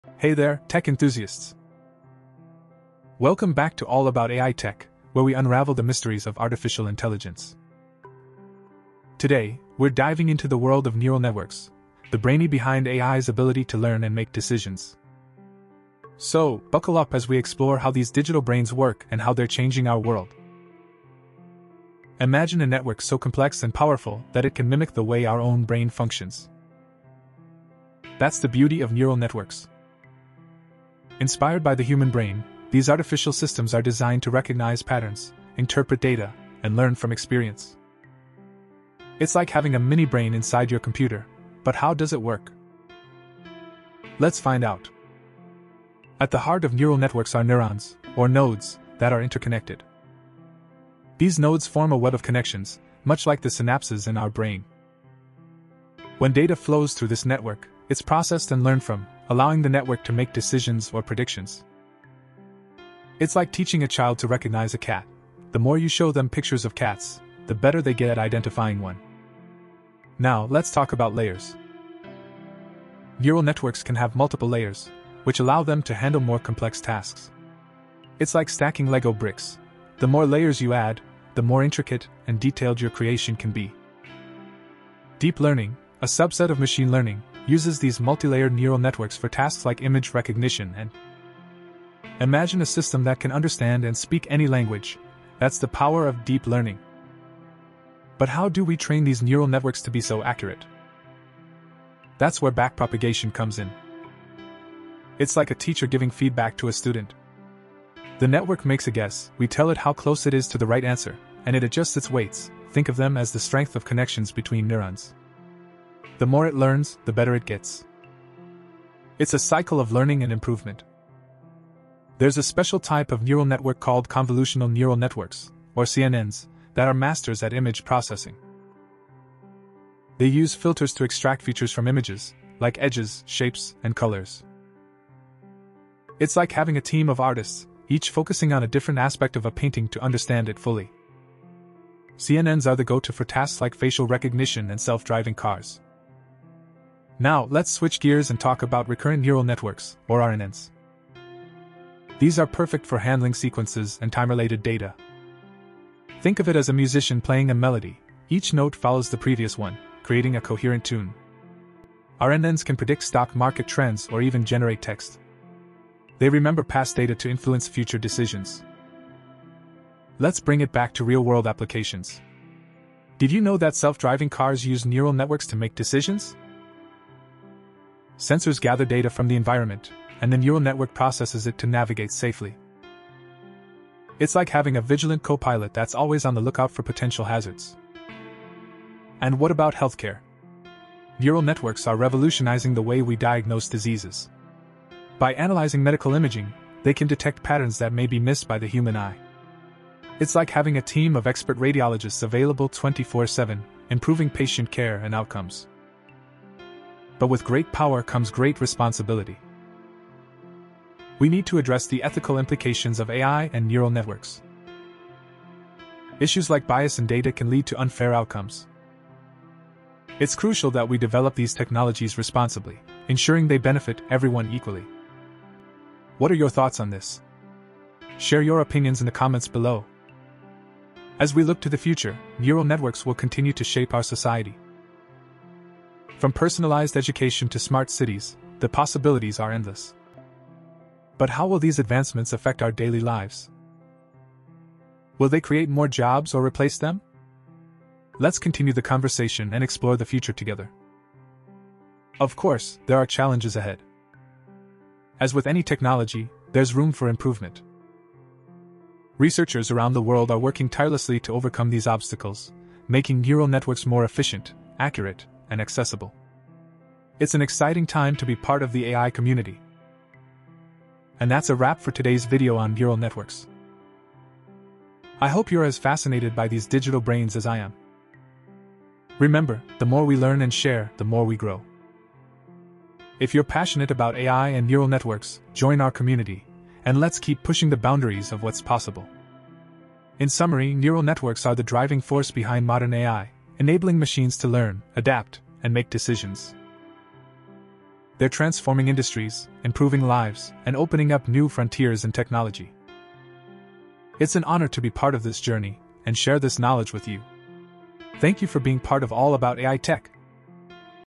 [Nerual Network]: What is Nerual Network
Neural networks are the foundation of modern AI, enabling machines to learn, adapt, and make decisions. Inspired by the human brain, these complex systems are composed of interconnected nodes that process and learn from data. Different types of neural networks, such as convolutional neural networks (CNNs) and recurrent neural networks (RNNs), handle specific tasks like image recognition and time-related data. Neural networks are revolutionizing industries, including self-driving cars and healthcare, but also raise ethical concerns such as bias. Researchers are working to overcome challenges and improve these technologies, shaping the future of AI and its impact on society.